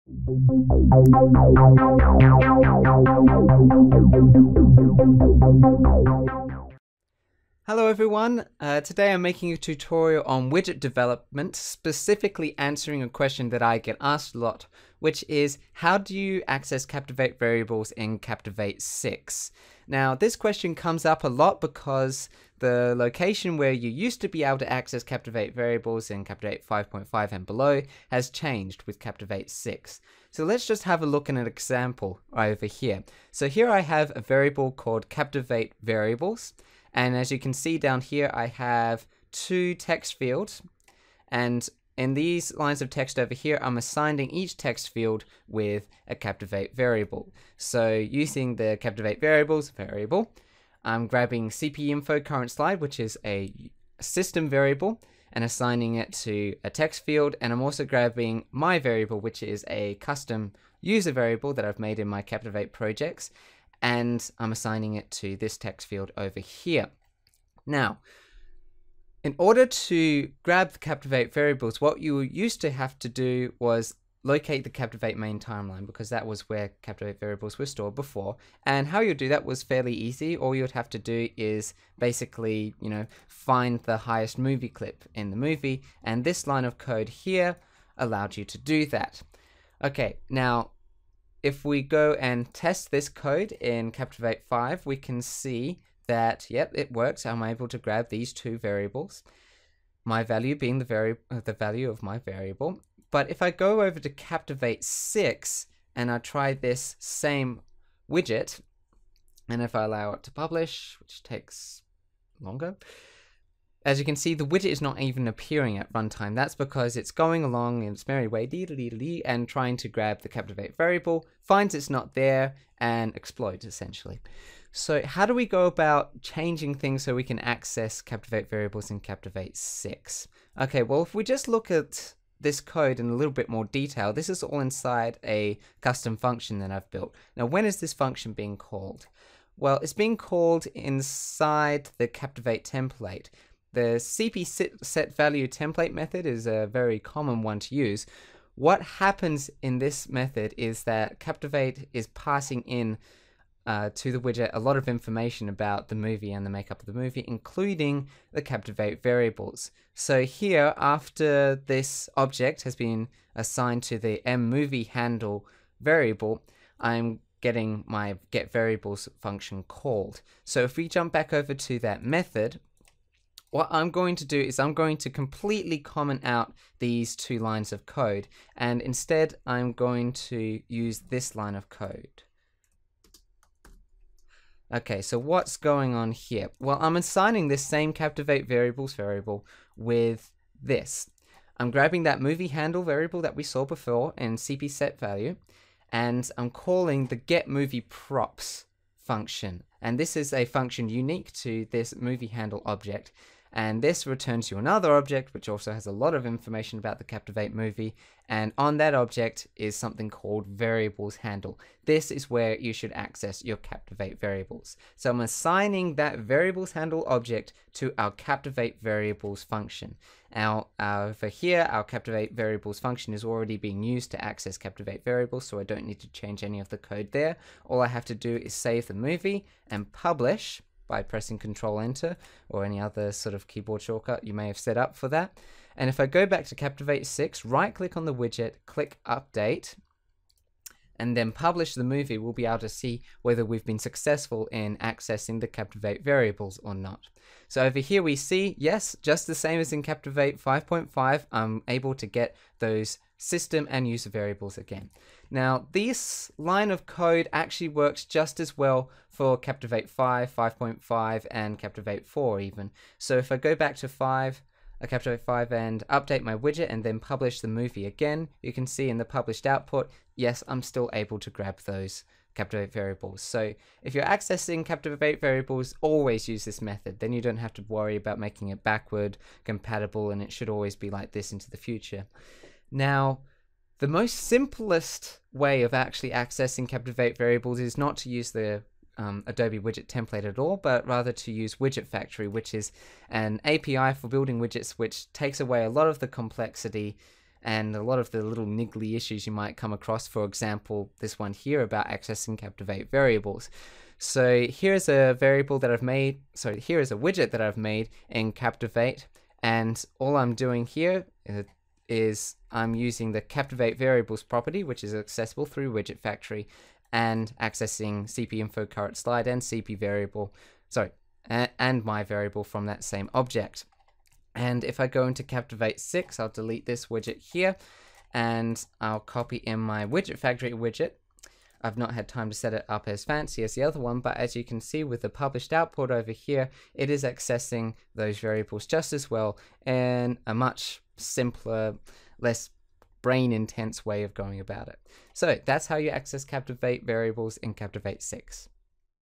Accessing Captivate Variables in Captivate 6
Tristan shows how to program a widget to safely access Captivate Variables for all versions of Captivate.